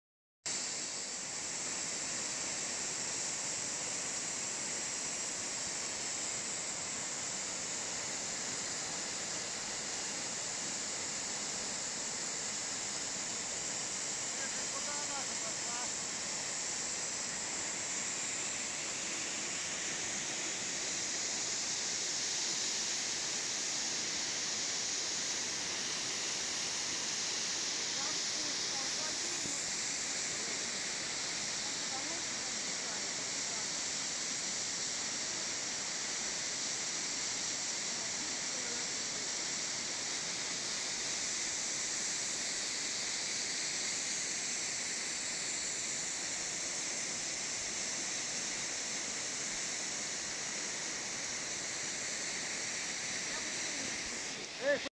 Водопад Vallaste - крупнейший в Эстонии
Валасте (Vallaste) считается самым высоким водопадом не только в Эстонской Республике, но и во всей Прибалтике. Его высота составляет 30,5 метров. Существует мнение, что водопад создали искусственным способом, преследуя цель отвести воду с сельскохозяйственных полей.